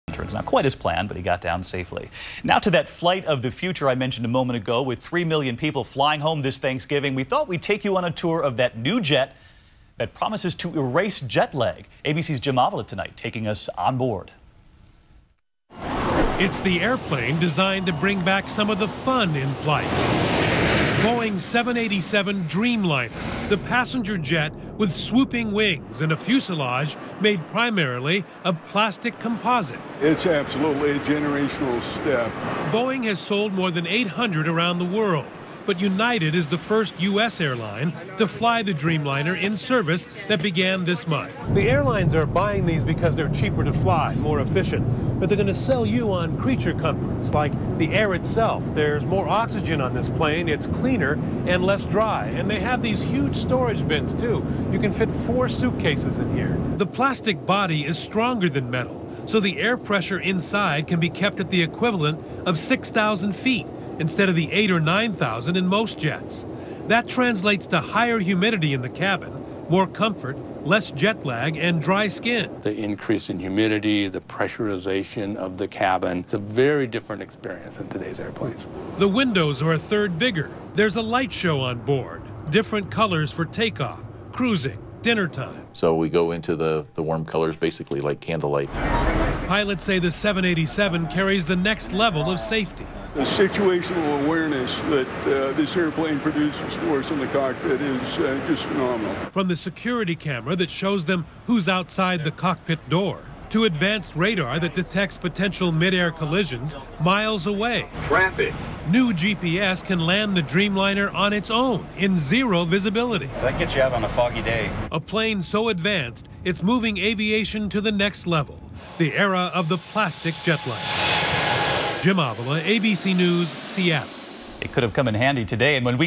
Modern Aircrafts (no more jet lag?)
ABCNews report on modern aircrafts now being deployed - designed for better comfort, lower pressurization means more moisture, less dry skin - erases jet lag, more... plastic instead of metal means stronger yet lighter - energy efficient.